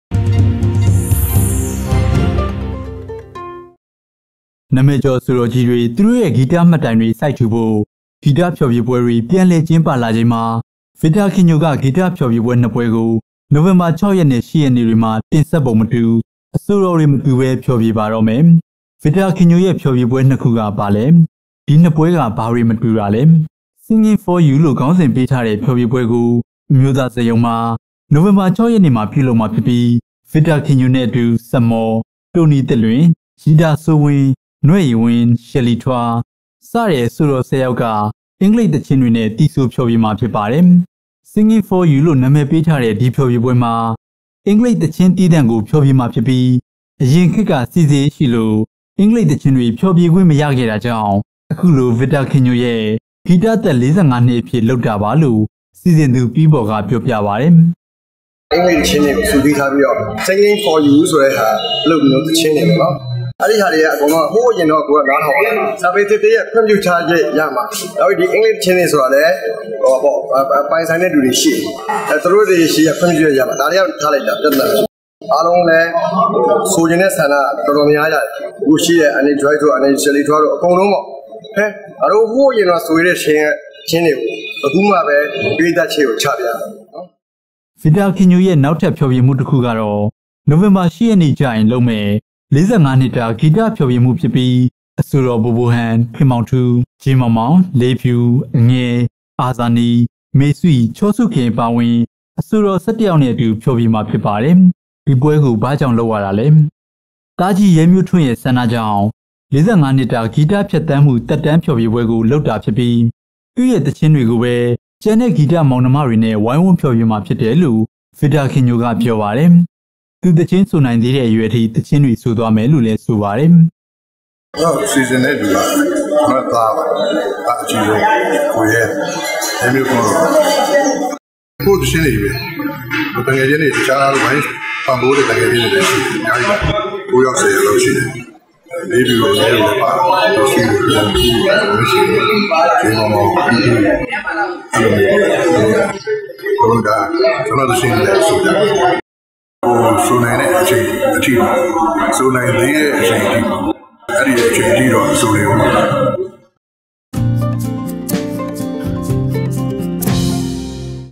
ဗစ္တာခင္ညိဳ ေဖ်ာ္ေျဖပြဲ ၂ခု၊ ၂ရက္ က်င္းပမယ္
နာမ​ည​္ေက်ာ​္​အဆို​ေ​တာ​္​ႀ​ကီး​ေ​တြ သူ​တို​႔​ရဲ​႕​ဂီ​တ​မွတ္တို​င​္ေ​တြ​ကို စိုက္ထူ​ဖို​႔ ဂီ​တေဖ်ာ​္ေျ​ဖ​ပြဲ​ေ​တြ ျ​ပန္လည္က်​င​္း​ပ​လာခ်ိန္မွာ ဗစ္တာ​ခင္ညိ​ဳ​က ဂီ​တေဖ်ာ​္ေျ​ဖ​ပြဲ ႏွစ္ပြဲ​ကို ႏို​ဝင္ဘာ ၆​ရက္နဲ​႔ ၈​ရ​က​္ေ​တြ​မွာ တင္ဆက္ပံု​မ​တူ၊ အဆို​ေ​တာ​္ေ​တြ မ​တူ​ဘဲ ေဖ်ာ​္ေျ​ဖ​ပါ​ေ​တာ့​မ​ယ​္။ ဗစ္တာ​ခင္ညိ​ဳ​ရဲ​႕ ေဖ်ာ​္ေျ​ဖ​ပြဲ ၂​ခု​က ဘာ​လဲ၊ ဒီႏ​ွစ္ပြဲ​က ဘာ​ေ​တြ​မ​တူ​တာ​လဲ။